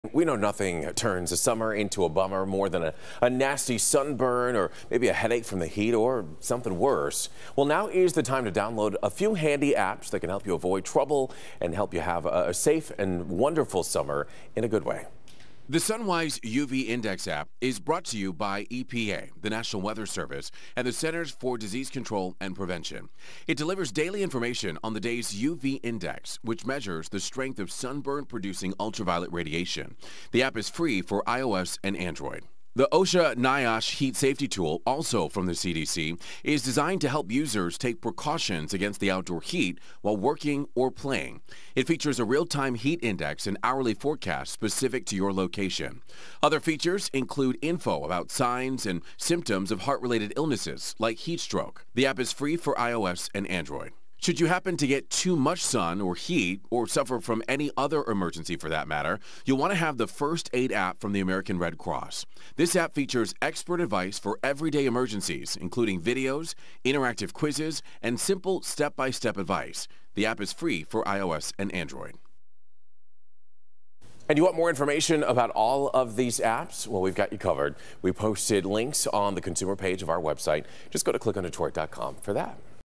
Sun safety apps: Staying safe in the sun
There are apps that can help you avoid trouble and have fun in the sun.